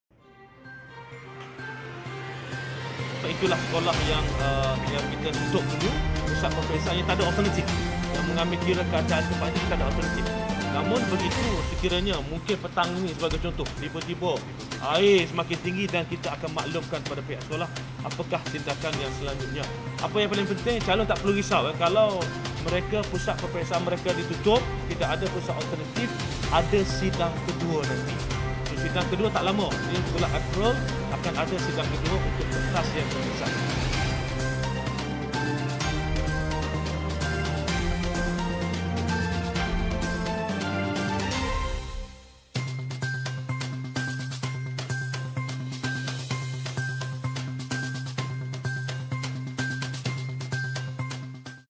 Banjir : 708 calon SPM di Pasir Mas tidak duduki peperiksaan hari ini
Susulan banjir yang menjejaskan tujuh sekolah di  Pasir Mas.

708 calon SPM dari jajahan itu dibenarkan menduduki peperiksaan kertas Bahasa Melayu dan Bahasa Inggeris pada sidang kedua, April depan.

Sekolah terlibat adalah Sekolah Menengah Kebangsaan (SMK) Rantau Panjang, SMK Gual Periok, SMK Baroh Pial, Sekolah Menengah Agama Maahad Muhammadi Rantau Panjang, SMK Meranti, Sekolah Menengah Ugama (SMU) Al-Falah Siram dan SMU Khairiah Banggol Setol.